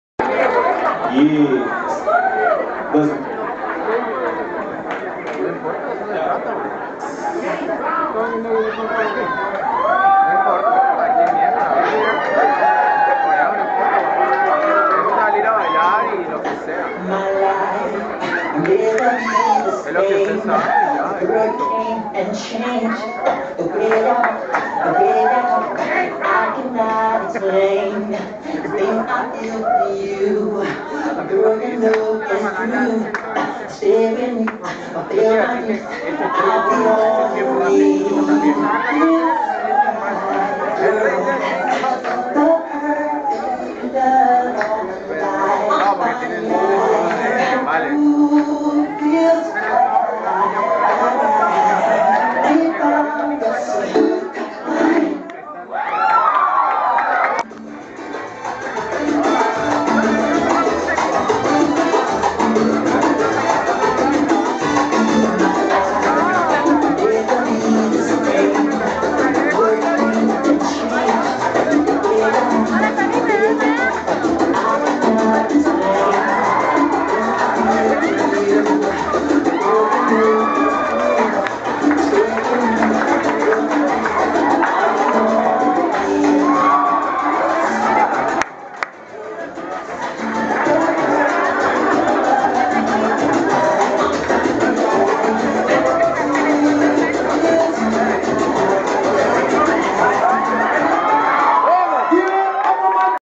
Stevens Steakhouse Coed Solo Contest 2012
Here is my performance at the Stevens Steakhouse Solo Contest. OMG. I am so crazy. I didn't ,make the finals but both host wanted me back.